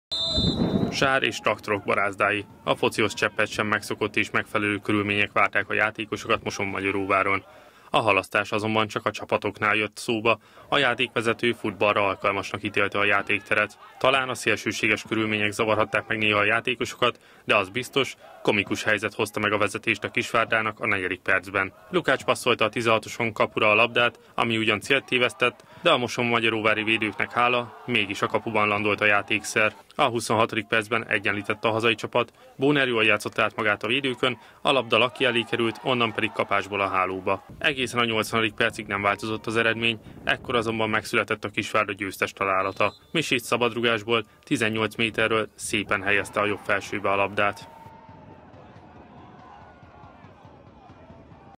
Idegenben száz százalék - Kölcsey Tv
Idegenben veretlenül zárta az évet a Kisvárda Master Good. A Mosonmagyaróvár otthonában lépett pályára vasárnap az NB II. 20. fordulójában Kondás Elemér csapata, ahol 2-1-es győzelmet aratott.
Győzelmével a Kisvárda megerősítette második helyét a tabellán, és 47 pontjával továbbra is két ponttal marad el az éllovas, hétvégén a Csákvár ellen 3-1-es sikert arató MTK mögött. Zárkózott a Szpari is. A Nyíregyháza Spartacus hazai pályán 3-2-re győzte le a Budafokot, és 33 ponttal a negyedik helyen áll már, mindössze egy ponttal lemaradva a dobogóról.